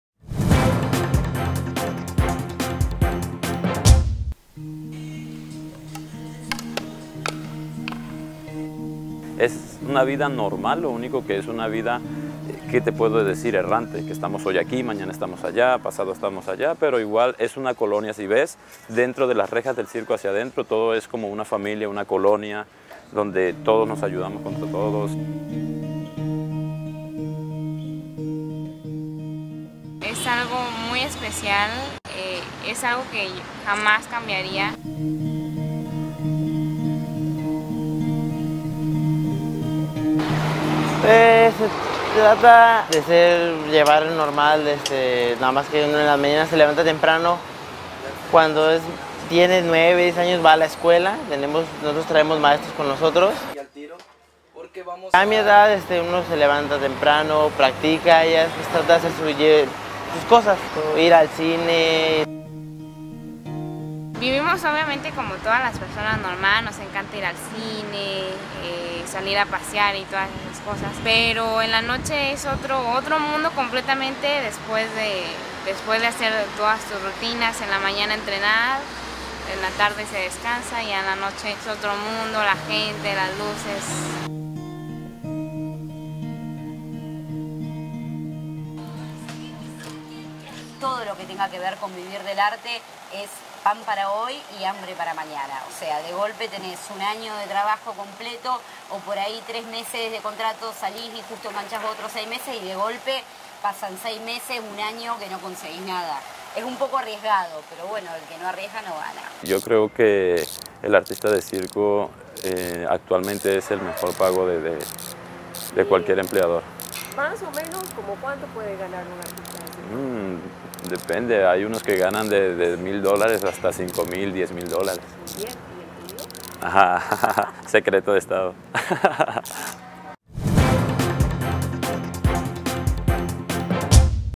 Artistas errantes
Ellos y ellas nacieron entre carpas y luces, viajan todo el año, regalan sonrisas, reciben aplausos, pero jamás se quedan ahí por mucho tiempo. Su vida es ser artistas errantes y así son felices.